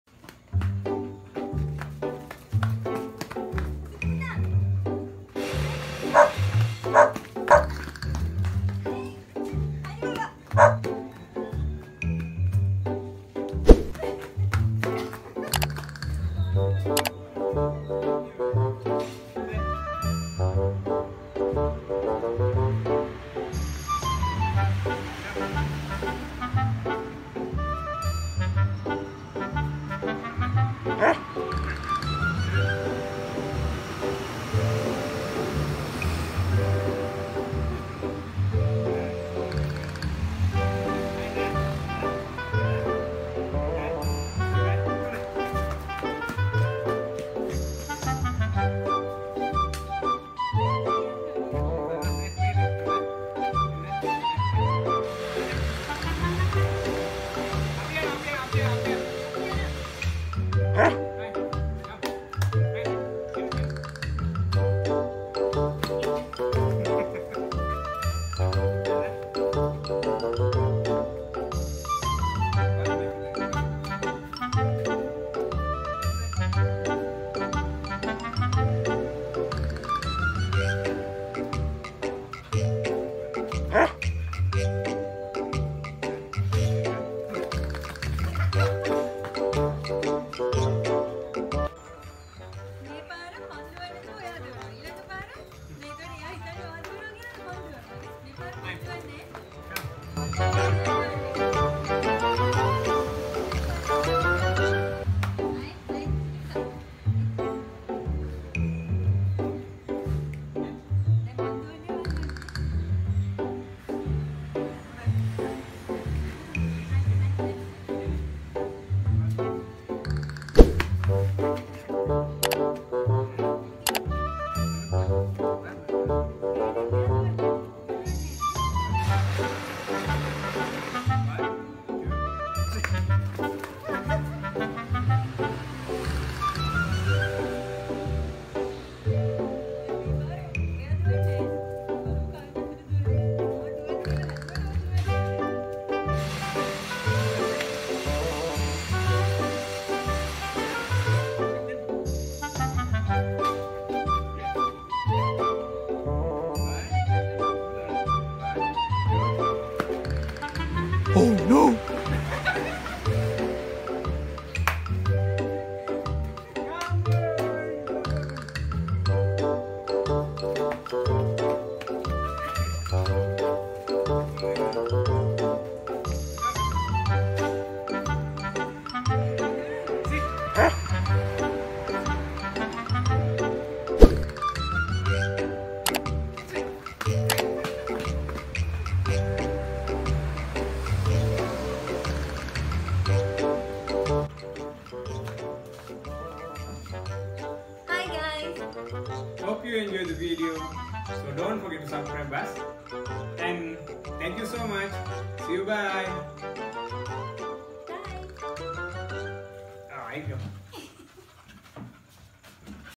Run In Opposite Directions To See Who Your Dog Loves More🏃🏼‍♀️ 🏃🏼🐕 |TikTok challange
Hello guys,  We tried the famous TikTok challenge "Run in Opposite directions to see who your dog loves more". Kylie was super excited and Let see who gonna win this challenge and who Kylie loves more.🤩